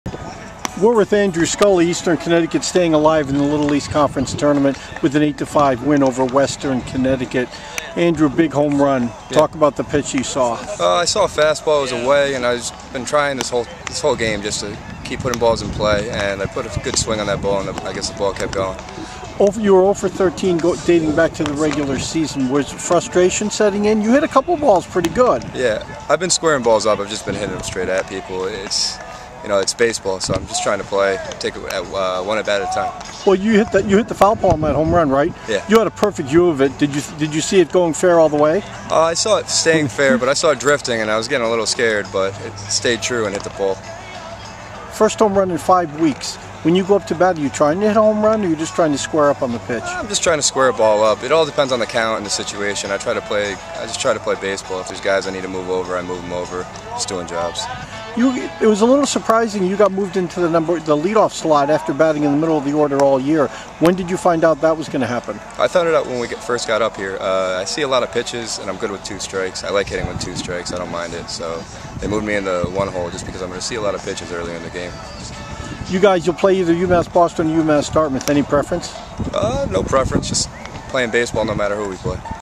Andrew Scully-Post-Game-May 7
Andrew Scully knocked a two-run home run off the right field foul pole to add two insurance runs in Eastern's 8-5 win over Western Connecticut in an elimination game of the 2015 LEC baseball tournament.